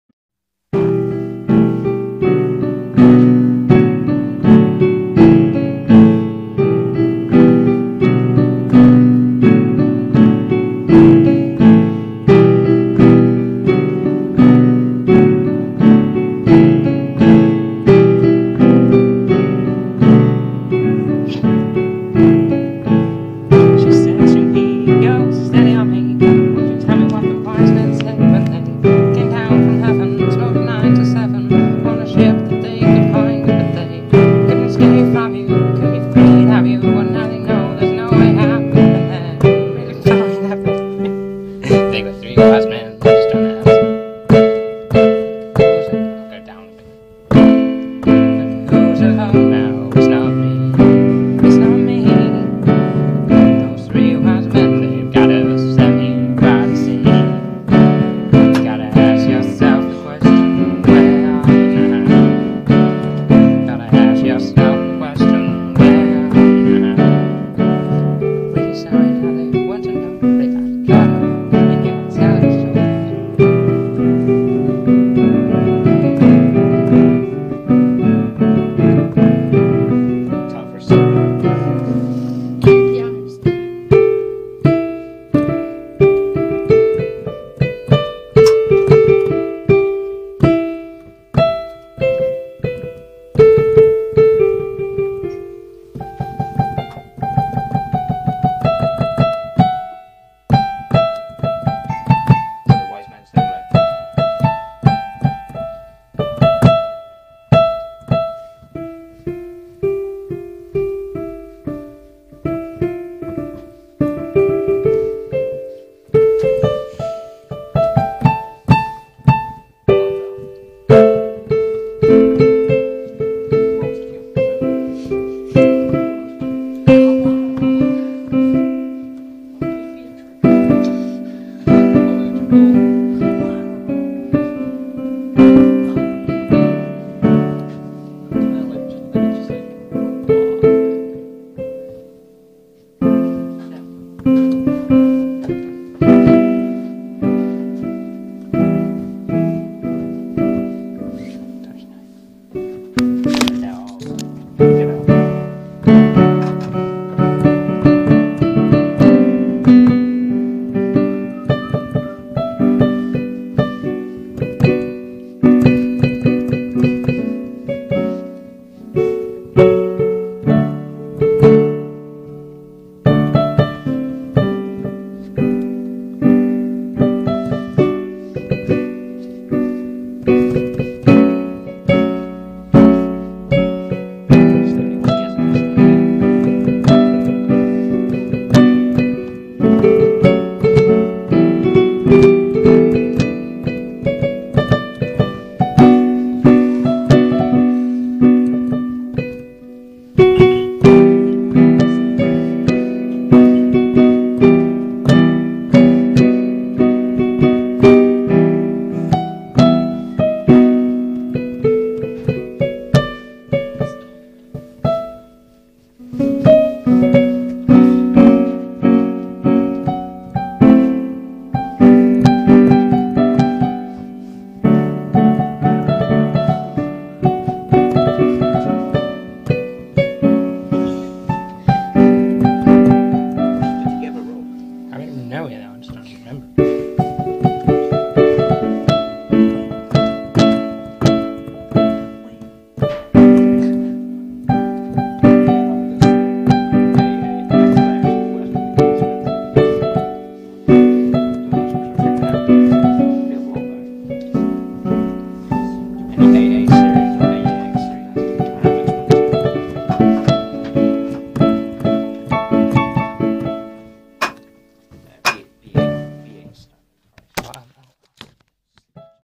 Rob Hannay playing Piano in Holmes Music
My good friend Robert having a go on a Piano in Holmes Music, Swindon.